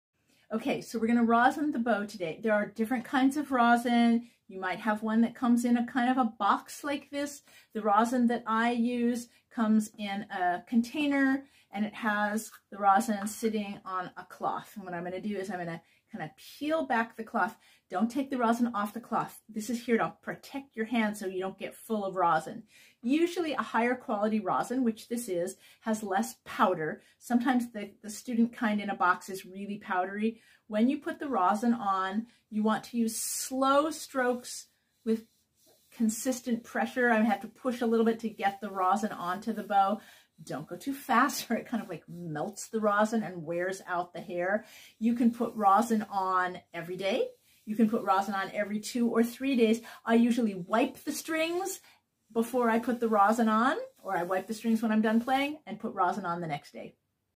How to put Rosin on your Cello Bow
Does and don'ts about when to rosin.